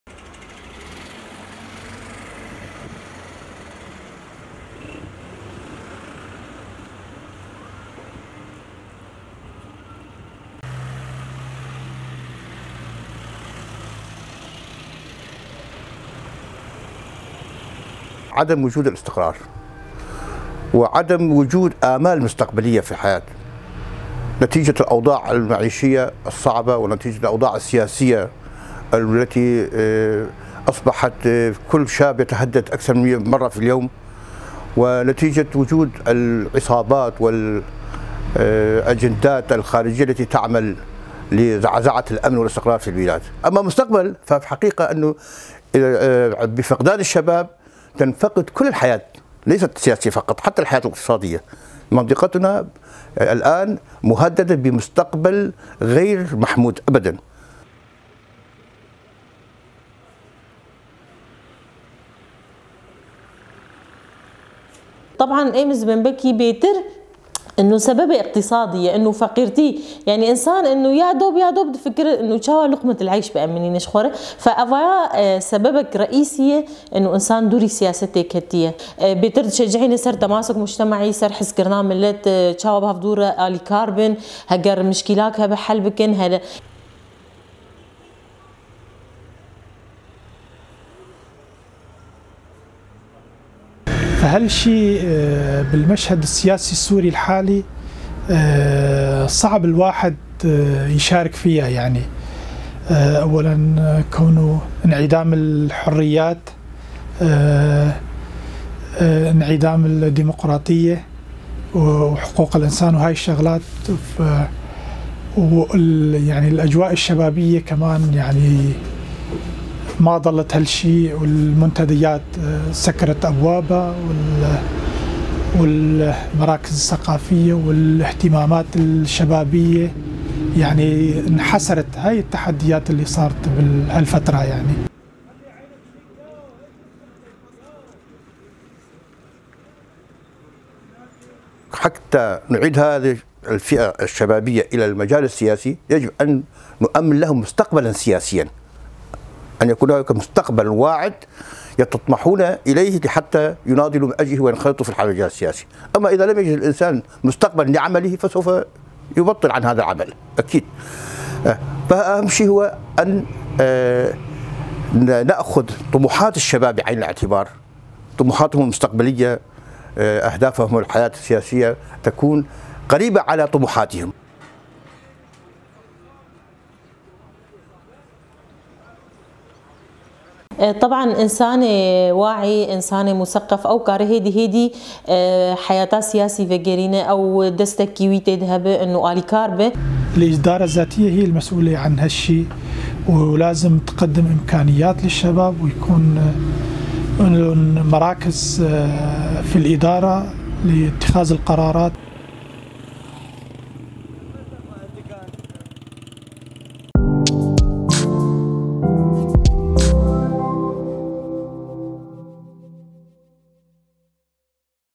عامودا: عزوف الشباب/ات عن المشاركة في الحياة السياسية
هذا العمل مبادرة مشتركة بين منظمة همزة وصل وشبكة آسو الإخبارية تسلط الضوء على عزوف الشباب/ات عن المشاركة في الحياة السياسية في شمال شرق سوريا.